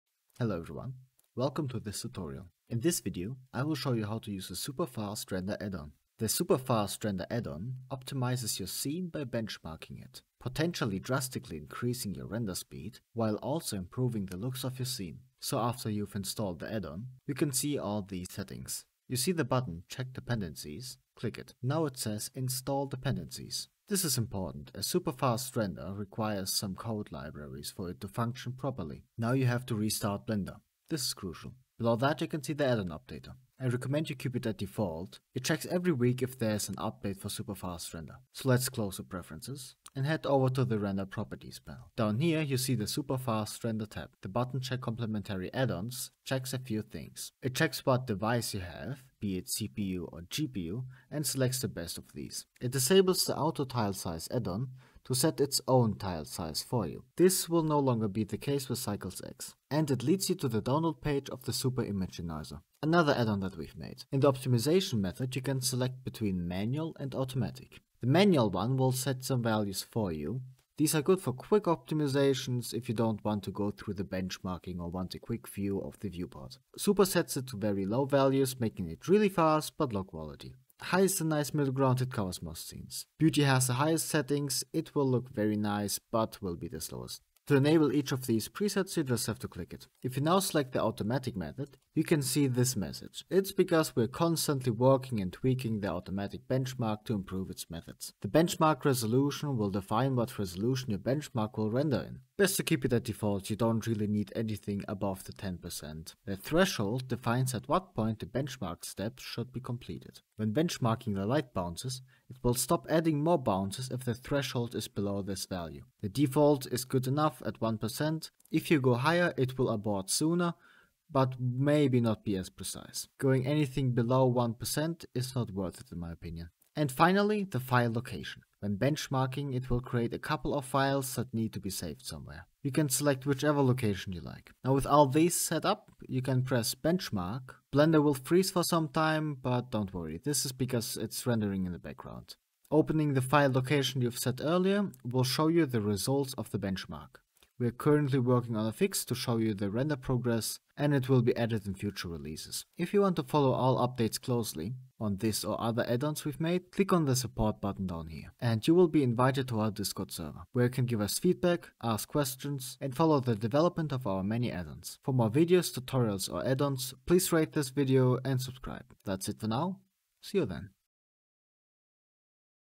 Super Fast Render v1.0.2 | Automatic Scene Optimization! - Blender 2.9+
Development of the Super Fast Render has continued!

If you want to see more of my work, here is my portfolio:

Do you have suggestions for further content like tutorials?
Tell me in my discord!

0:00 Intro
0:15 Preferences
0:40 Render Properties
1:05 Manual Setup
1:30 Automatic Setup
2:30 Outro